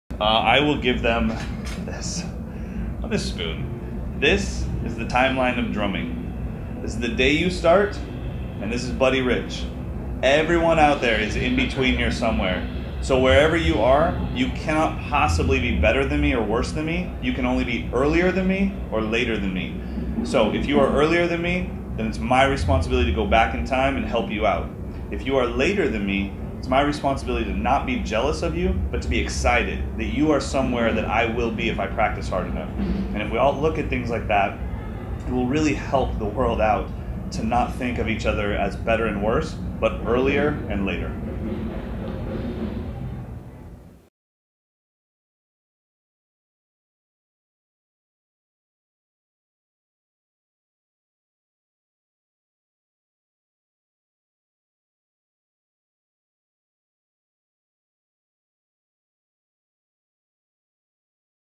Mike Johnston gives the best drum lesson for free
- The Mike Johnston interview - Part 9:
When asked for a personal note to everyone out there, Mike gives one of the best lessons you can think of. And not only for learning how to play drums.